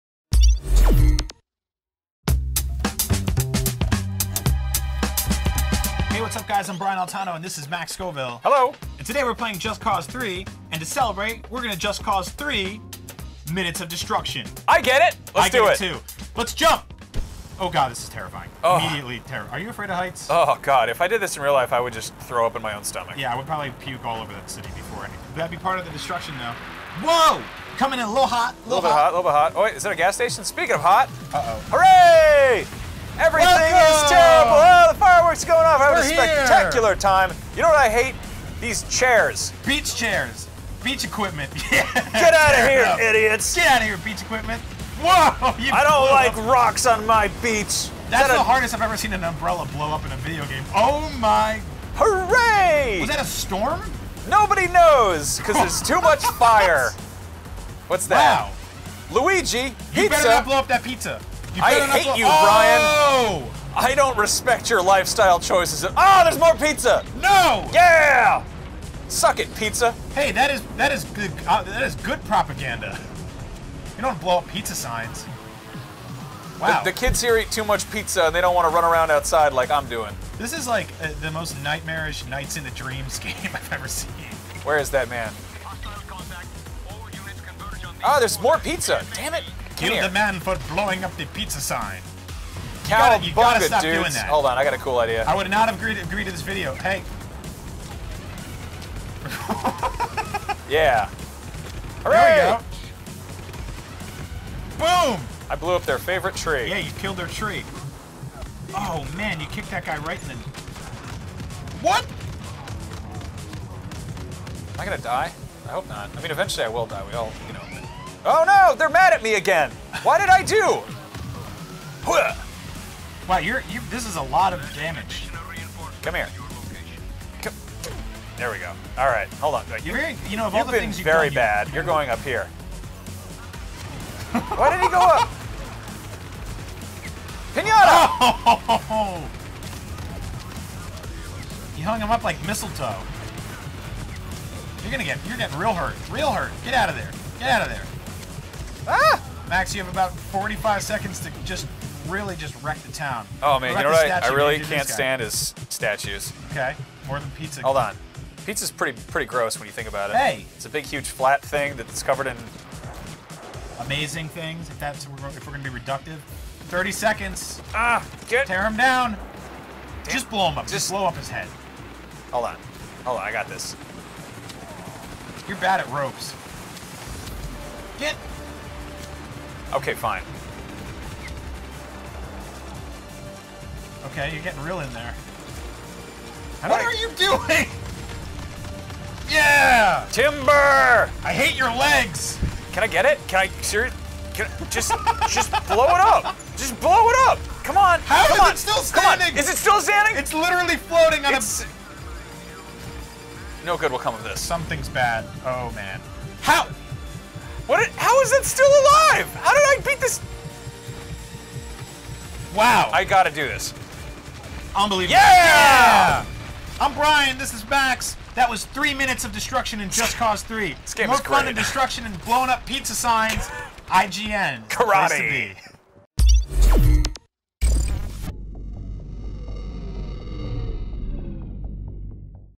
IGN
¿Han ido alguna vez a la Isla de Medici? Nos contaron que ahora lo puedes hacer en 3 minutos.